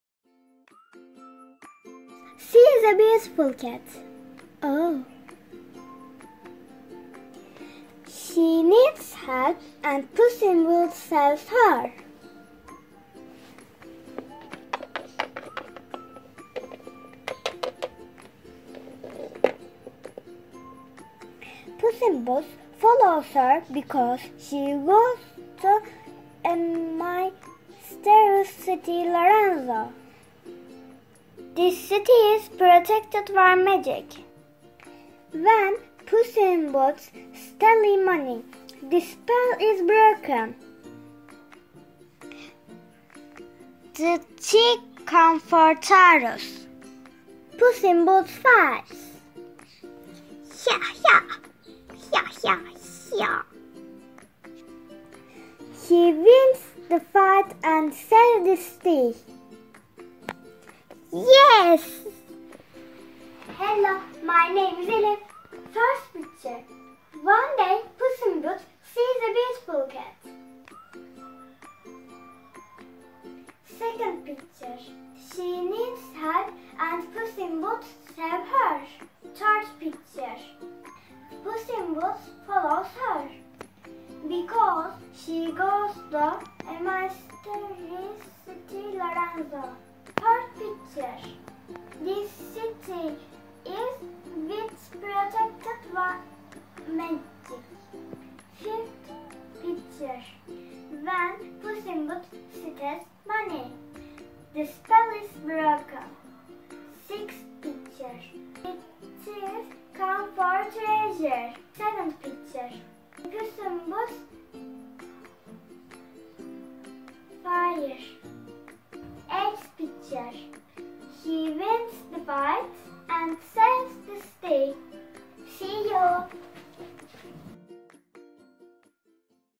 4B ELİF ADA ÜNER/ PUSS IN BOOTS- STORY WHEEL
It’s time  to retell our story ‘ PUSS IN BOOTS’ with a story wheel.